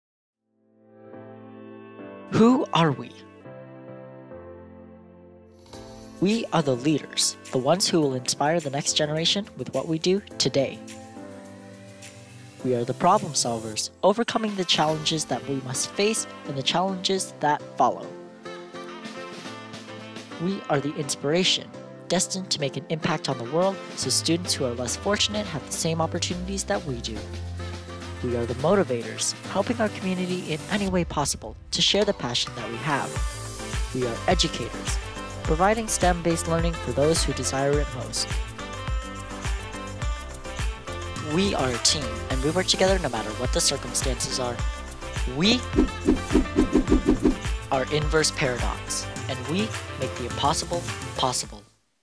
Inverse Paradox Promotional Video 2019
FRC Team 1325's promotional video for 2019

This year, we managed to find a home and we became Mississsauga's Community robotics team!